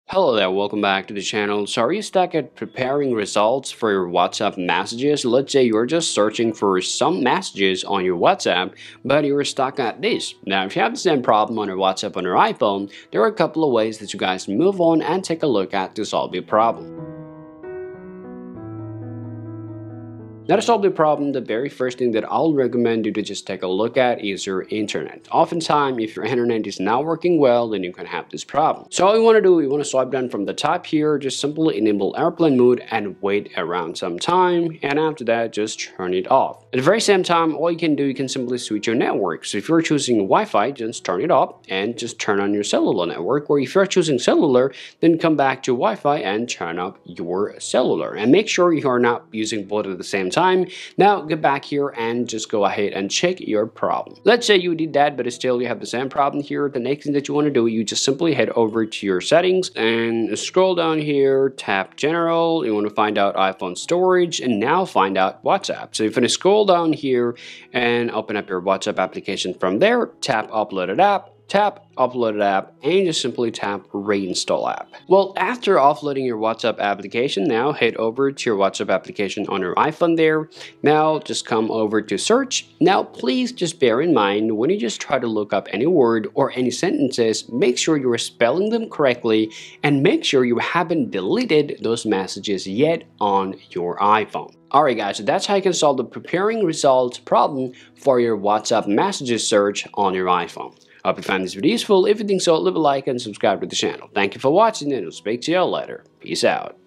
Fix- WhatsApp Messages Search Not Working on iPhone! [It Says Preparing Results 0%]
WhatsApp Chat Search doesn’t work for old messages on iPhone and got stuck on ‘Preparing result 0%, 4%, 3%, 6%, 9%? The 2 solutions to fix unable to search WhatsApp messages problem effortlessly. This issue often occurs after restoring the iCloud WhatsApp backup on your iPhone.

We will also show what you must keep in mind while searching for anything on WhatsApp Chat to avoid any issues in the future.

0:00 Common WhatsApp Search not working Problem
0:20 Solution 1: Check and Fix Internet
0:56 Solution 2: Offload WhatsApp
1:23 Keep in Mind while Searching something on WhatsApp